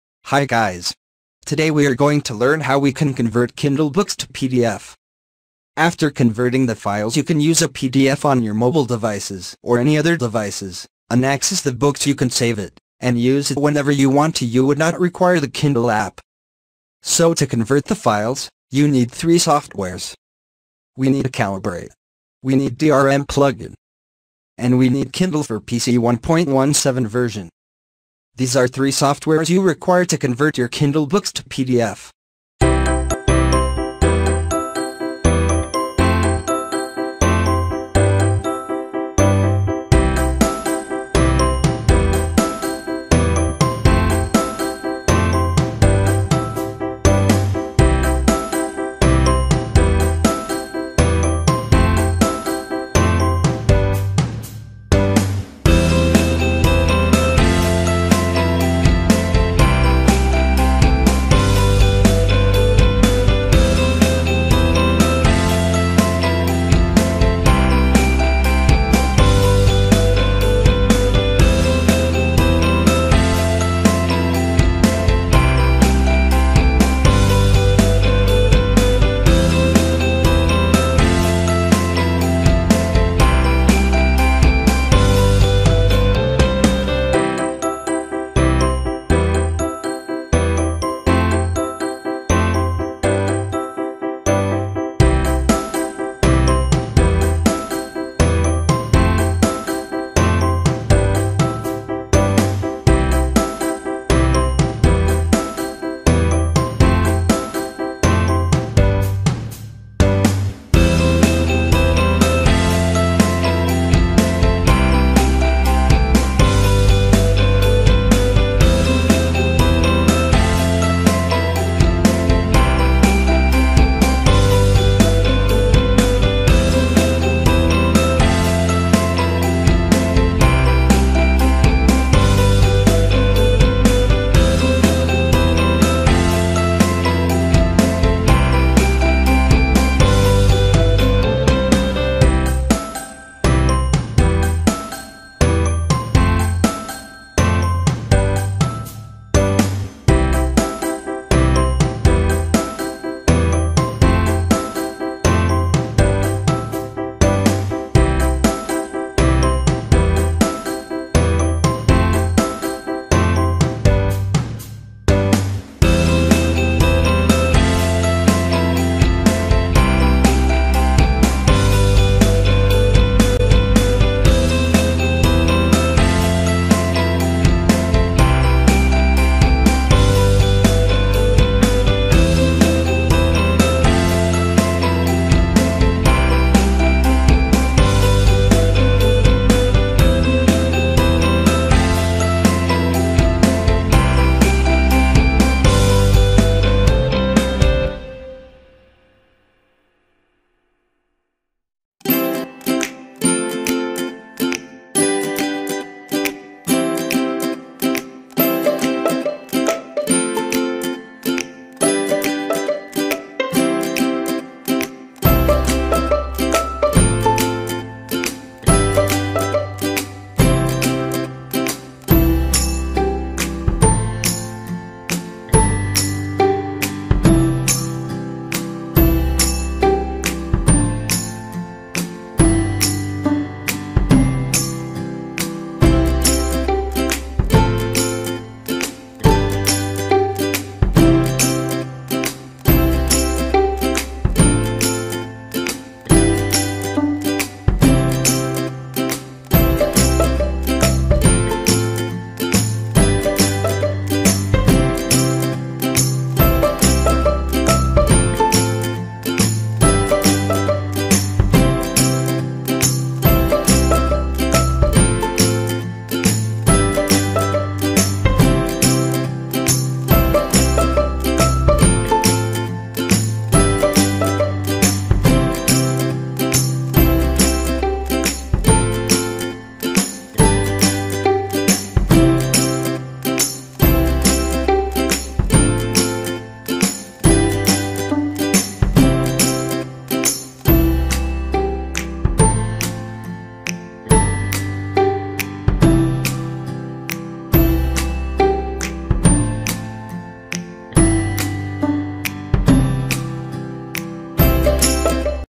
How to convert Kindle Books to PDF using Calibre
This video will show you how we can convert Kindle books to PDF using free tool.
Calibre is software specifically designed to transfer and alter digital text into various formats. This software can support countless input formats, including MOBI, HTML, PRC, and AZW.